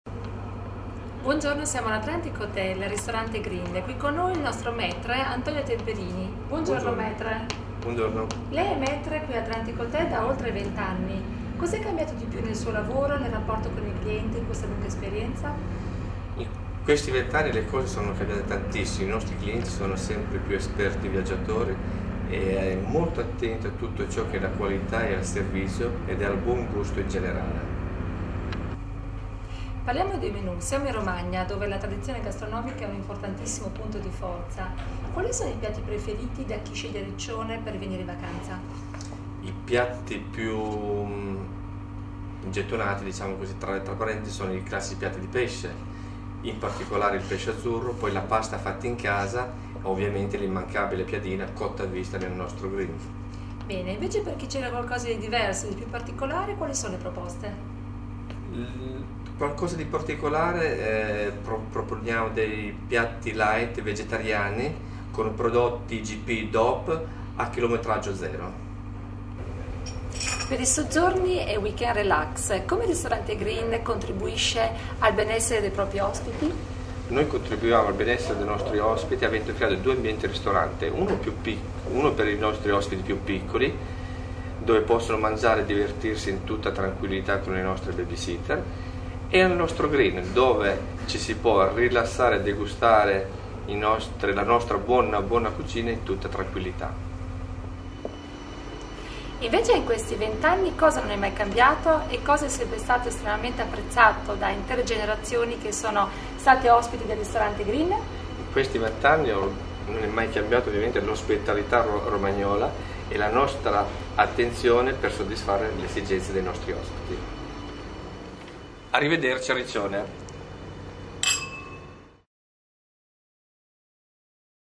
3 Domande a: Antonio Temperini, Maitre al Ristorante Green, Atlantic Hotel di Riccione
Eleonora Cavalli intervista il maitre Antonio, che da oltre 20 anni sovraintende al Ristorante Green dell'Atlantic Hotel di Riccione